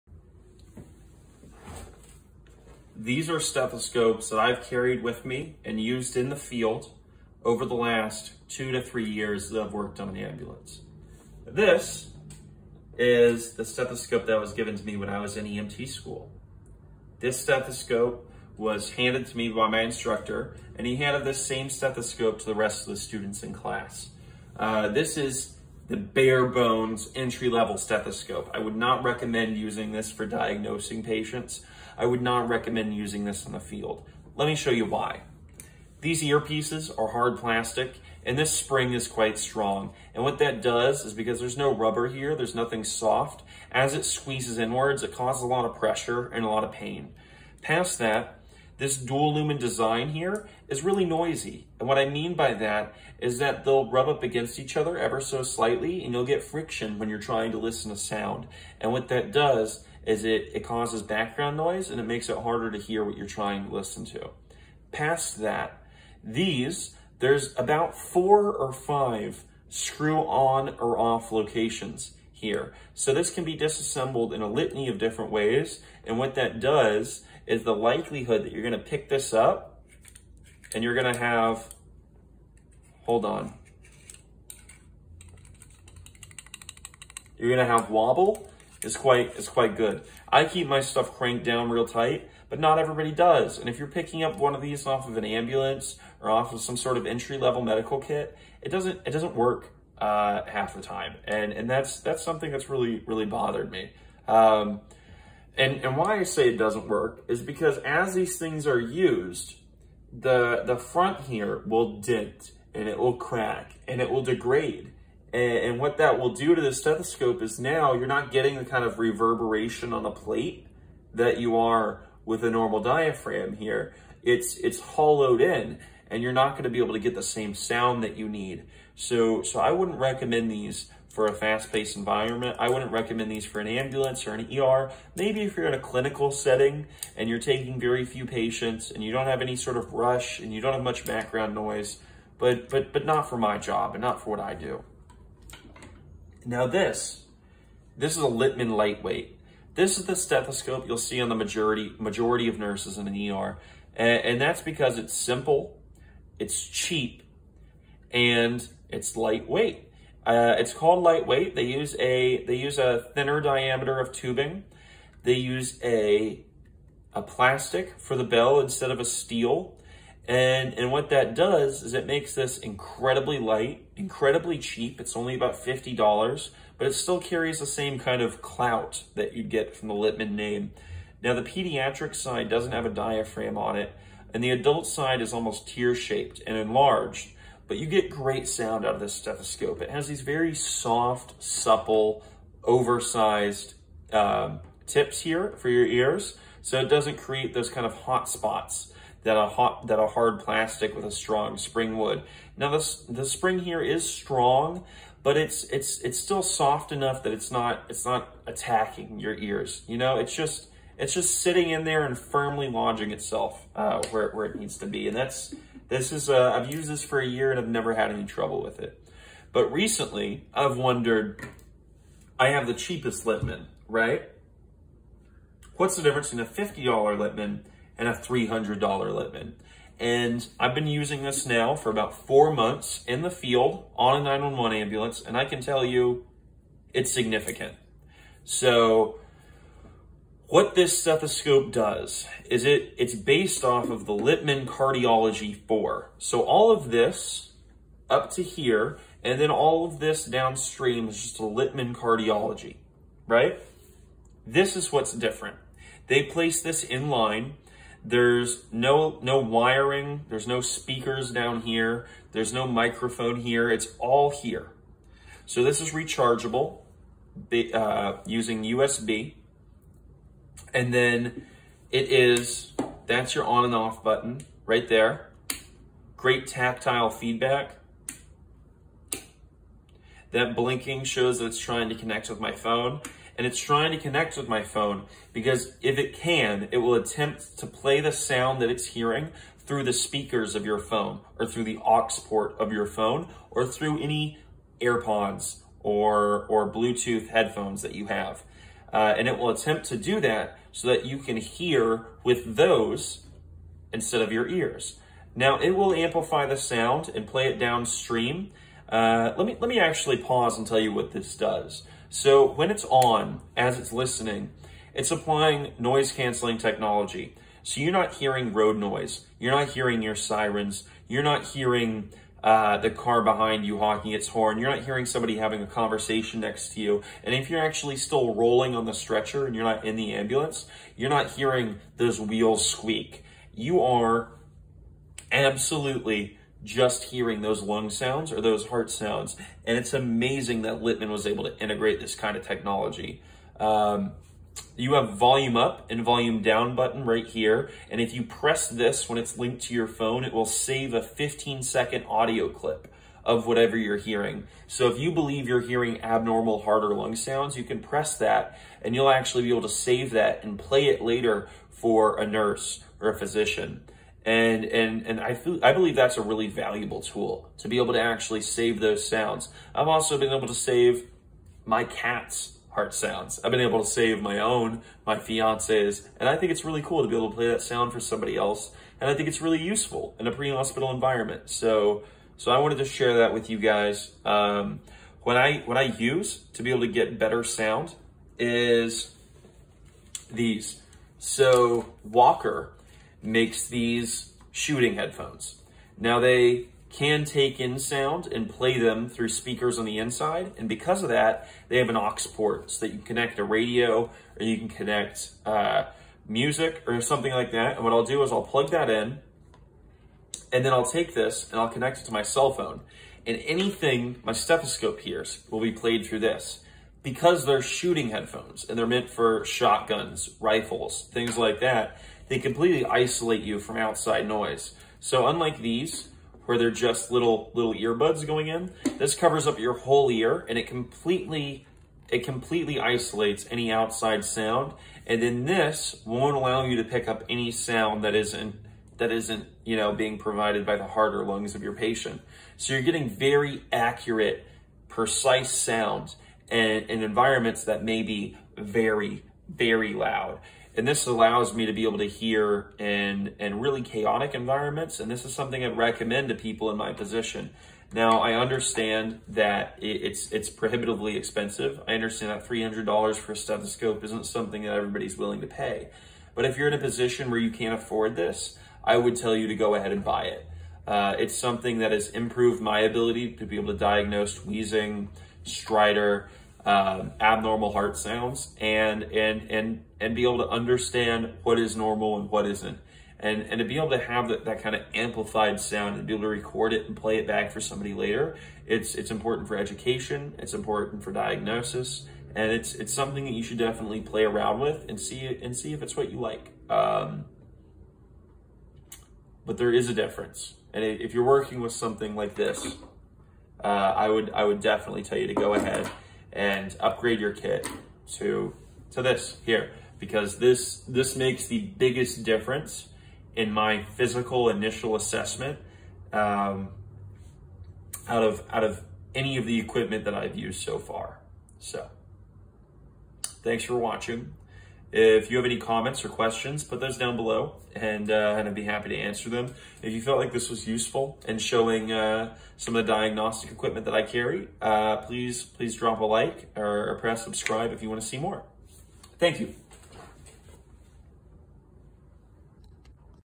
Stethoscopes for the EMT
Included in the video are the:

Littmann lightweight
Littmann CORE

I would list the other stethoscope if the manufacturer bothered to put a name on it.

I’m not recommending that every EMT become RoboCop, but stethoscopes have not changed in a very long time. I think it’s time for an update, don’t you?